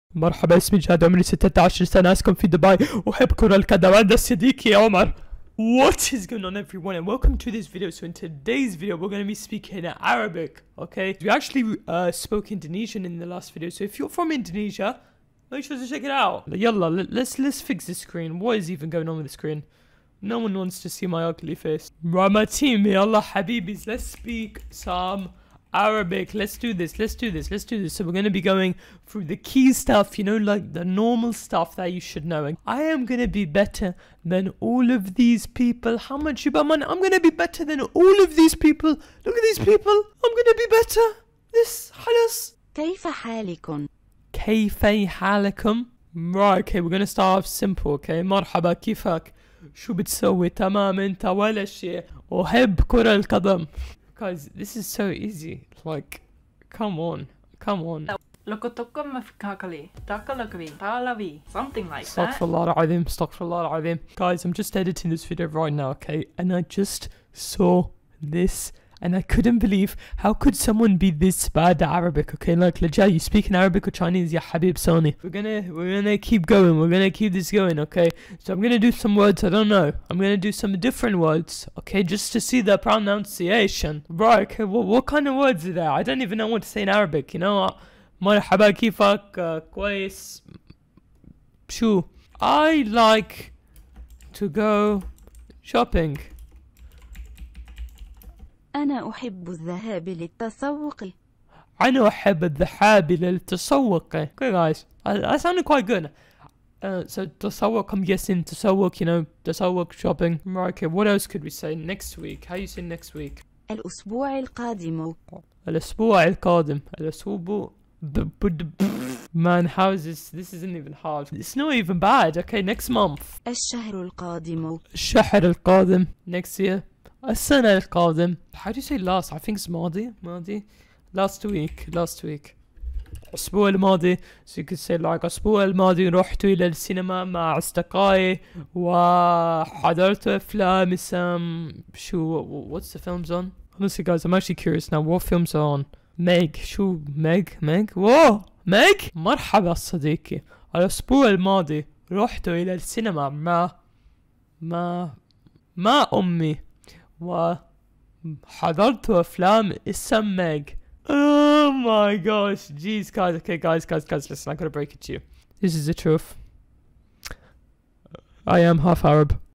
ENGLISH BOY SPEACKING ARABIC FOR FIRST TIME!
Welcome to this video of me (who is English) trying to speak Arabic for the first time! I love Arabs and love the language (Arabic). Thank you to all my Arab subscribers for suggesting I try to speak Arabic :)
►I make loads of funny videos, ranging from gaming videos to reaction and IRL videos!